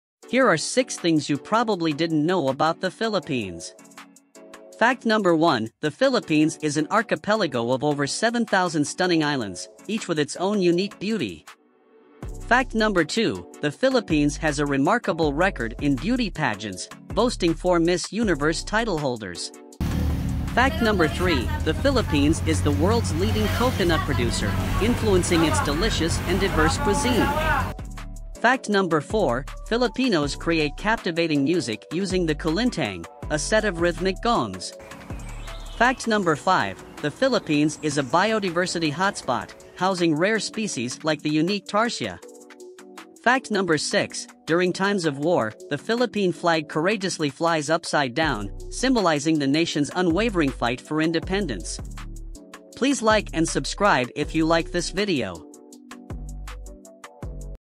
5 Things You Probably Didn't Know About the Philippines!
In this captivating video, we dive into the incredible wildlife and biodiversity of the Philippines. Join us on a virtual journey as we explore the hidden gems and rare species that make this country a natural wonderland. Discover the fascinating world of the tarsier, a small primate with big eyes, and learn about the majestic Philippine eagle, an endangered species symbolizing the country's conservation efforts. From the critically endangered Tamaraw to the stunning Palawan Peacock-Pheasant, we showcase the rich diversity of the Philippines' flora and fauna. Immerse yourself in the breathtaking landscapes and untold stories of this biodiversity hotspot. Get ready to be amazed by the beauty and wonders that the Philippines has to offer!